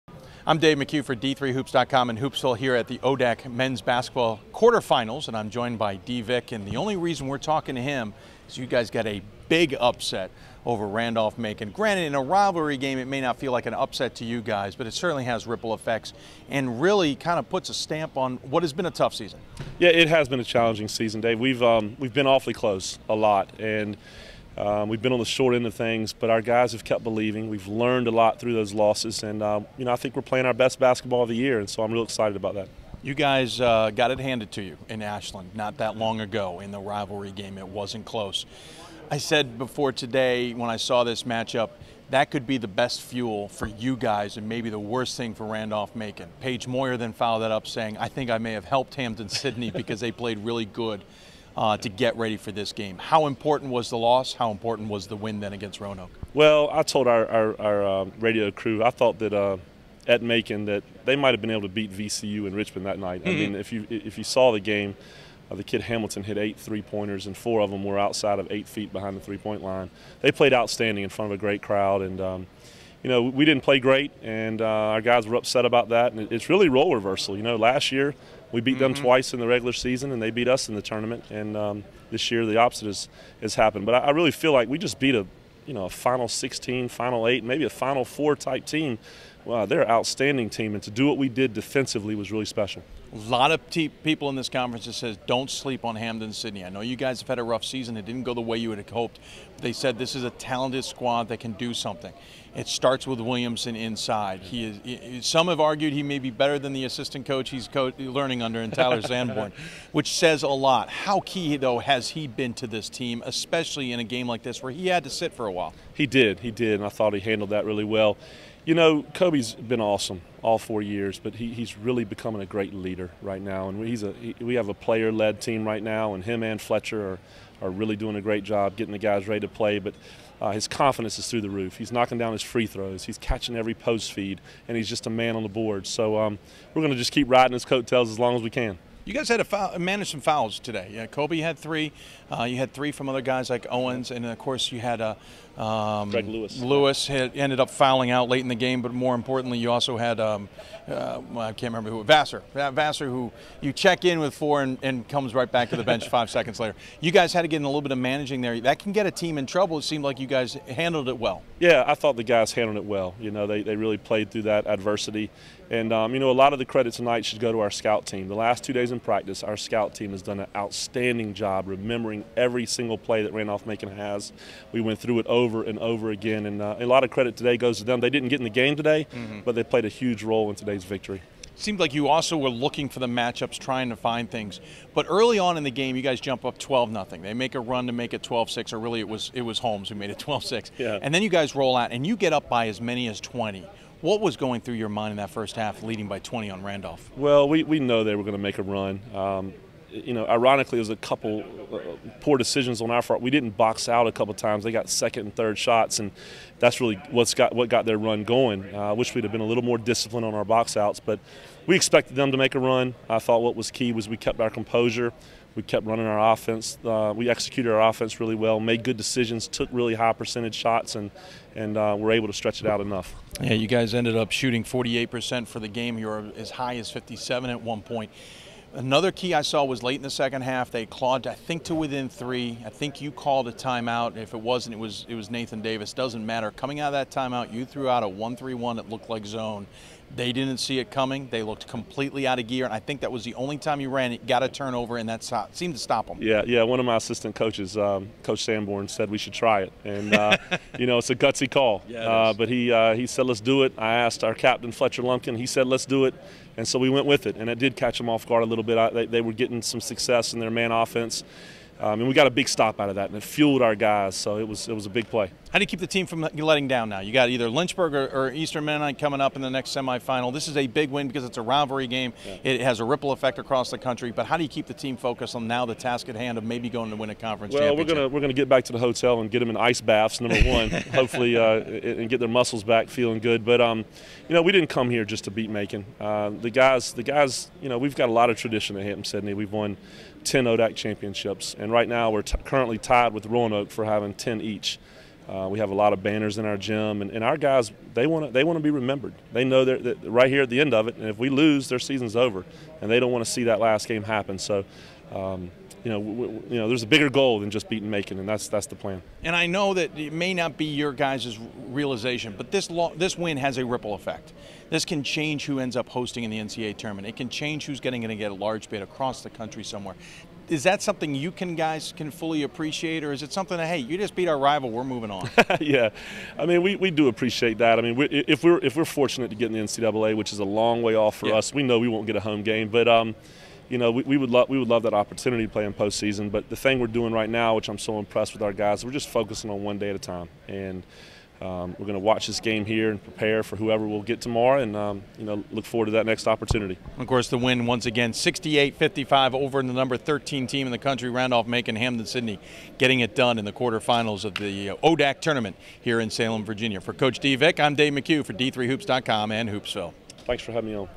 Hoopsville: Hampden-Sydney's Dee Vick
D3hoops.com and Hoopsville's Dave McHugh talks with Hampden-Sydney men's basketball coach Dee Vick after the team's 68-55 upset of #13 Randolph-Macon in the quarterfinals of the ODAC tournament in Salem, Virginia. Coach Vick talks about how the team executed their plan and offense despite the eventual run by RMC to claw their way back into the game. He also talked about how a win like this and a chance for a conference title may help forget a challenging season. Vick also discussed how important Khobi Williamson has been for the program.